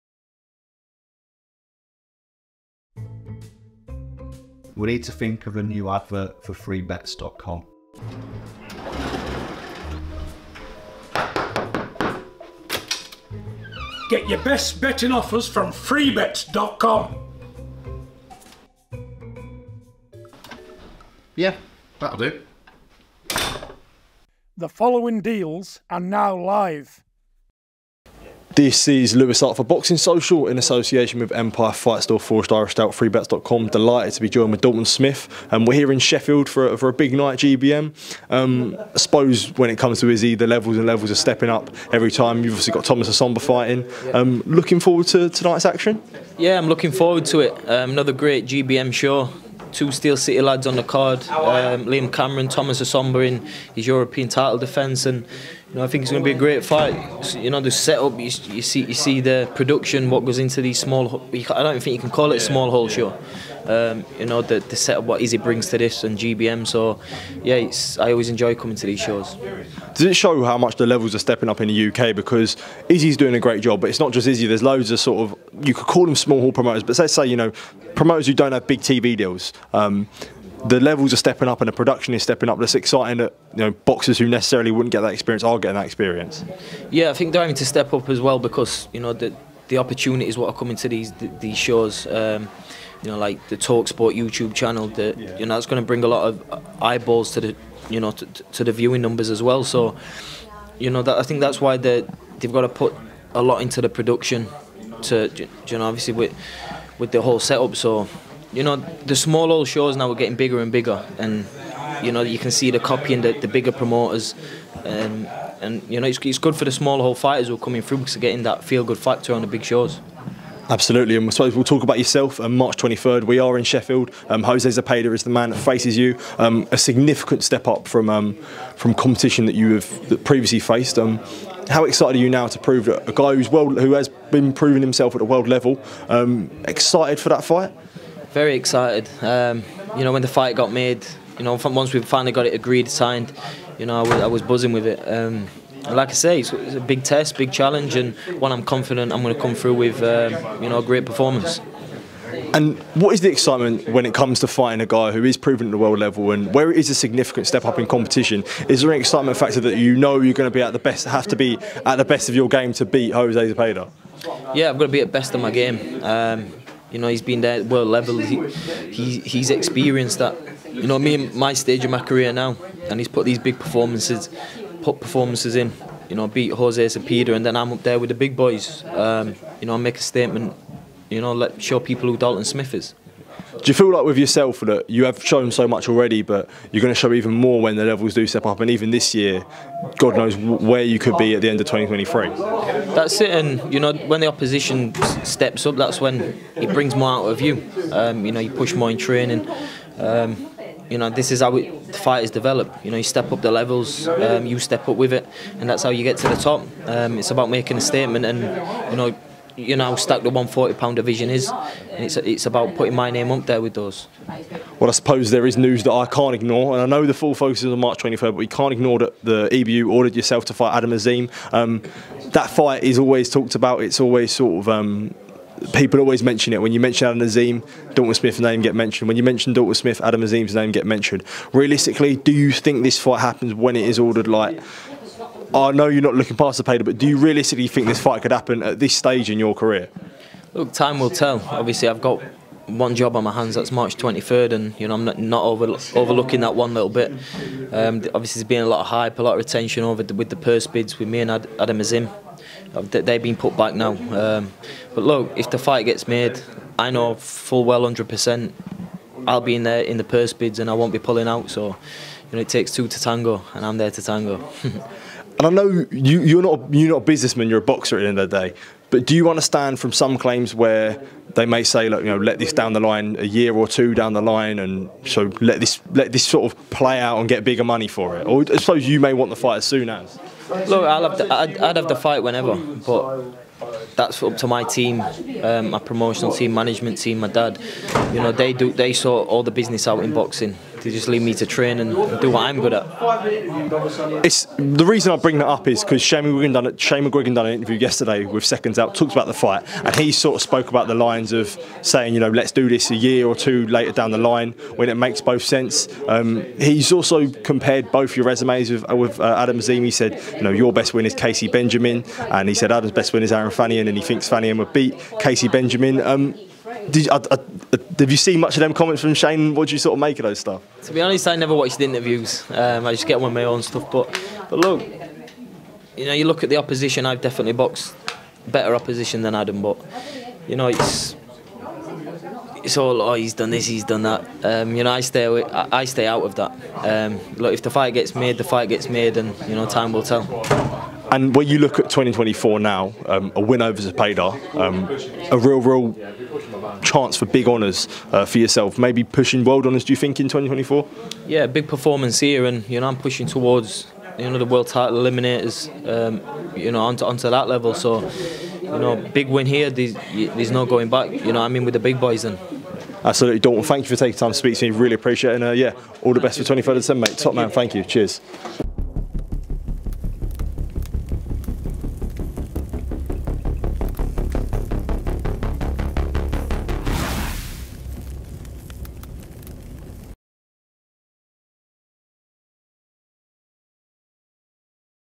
Dalton Smith On Adam Azim Clash: “I Won’t Be Pulling Out Of Purse Bids!”
Boxing Social's Louis Hart spoke to Dalton Smith, in association with Forged Irish Stout as they spoke about boxing in Sheffield for GBM, where they spoke about the show where Dalton also looks forward to headlining in Sheffield. Dalton reacted to being ordered to face Adam Azim for the European Title, he reacted to comments made by Shane McGuigan.

0:00 Intro
0:30 GBM Show In Sheffield
2:02 Jose Zepeda Fight
2:40 Step Up In Competition
3:45 Does The Adam Azim Fight Happen?
5:10 Shane McGuigan Comments
6:00 Ending

🍻 | Boxing is better with Forged Irish Stout! Buy 24 cans and get your FREE Forged glass